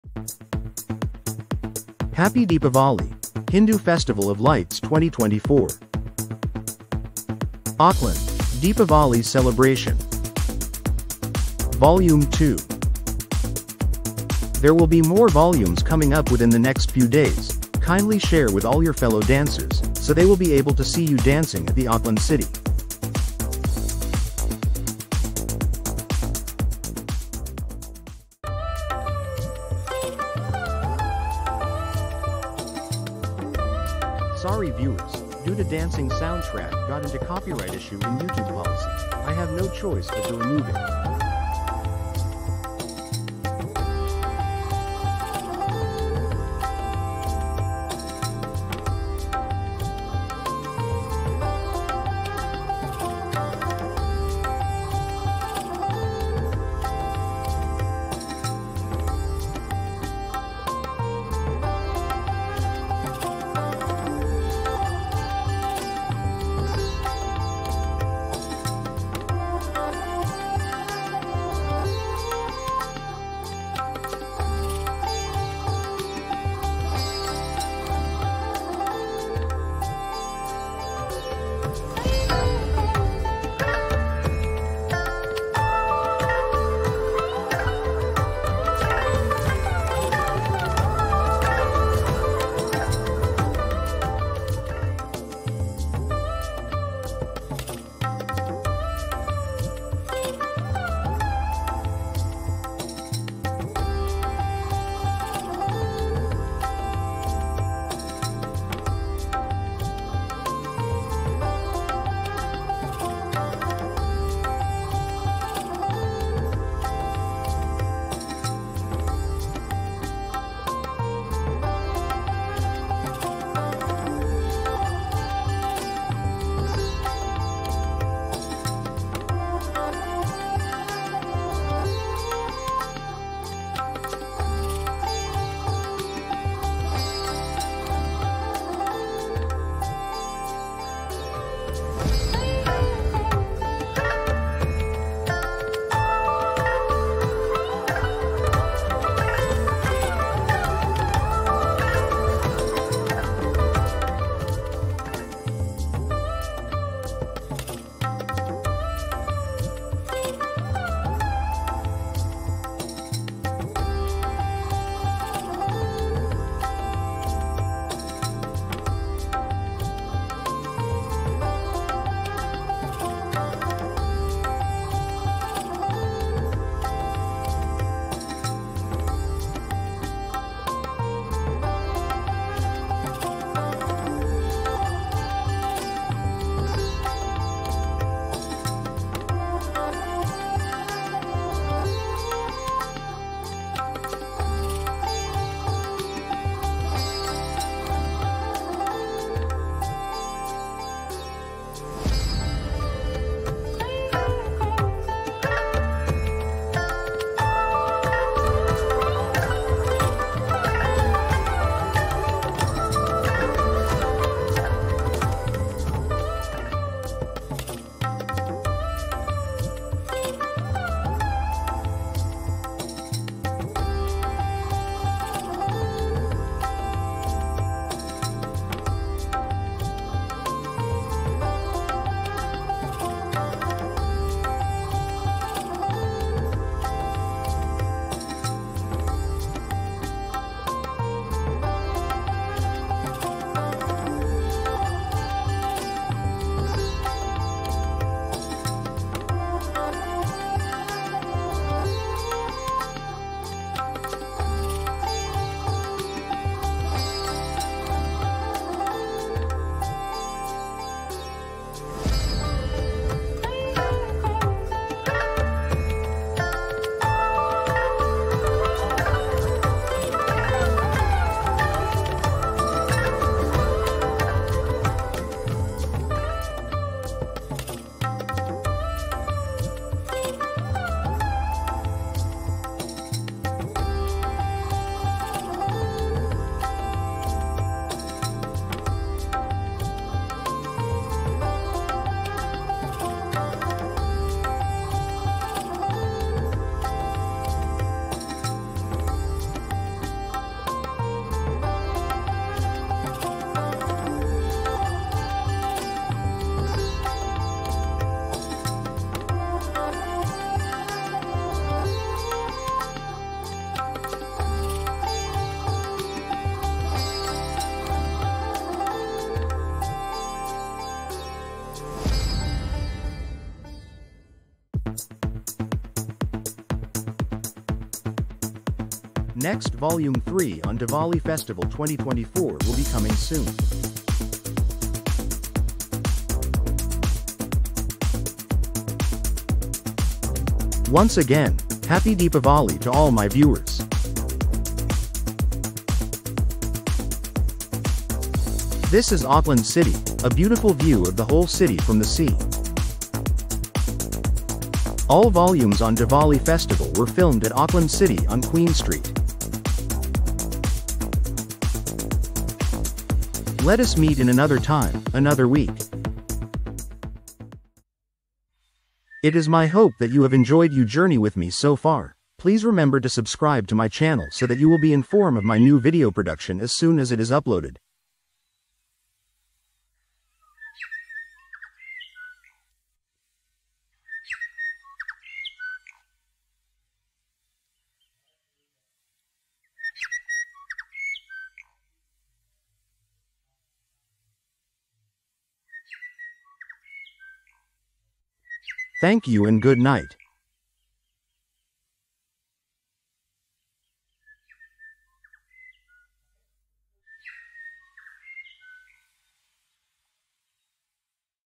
Auckland Diwali Festival (Vol. 2) 2024
Today I am going to Wish all my 175K Viewers " A Happy Deepavali 2024 "
I was fortunate enough to be at the Diwali Festival at the Auckland City Centre at Queen Street on the 19th October, 2024. I managed to film all the dancing performers so I will show them from Vol.1 to Vol.20 and hopefully I can still get them done within the next few days while the Diwali Festval in New Zealand still on. Wishing you all "A Happy Deepavali. "
Diwali, also known as Deepavali, is the Hindu festival of lights, with variations celebrated in other Indian religions. It symbolises the spiritual "victory of light over darkness, good over evil.

I will be uploading more videos that was filmed at Auckland City 2024...upcoming soon ( Vol.3 )

Once again, Happy Deepavali !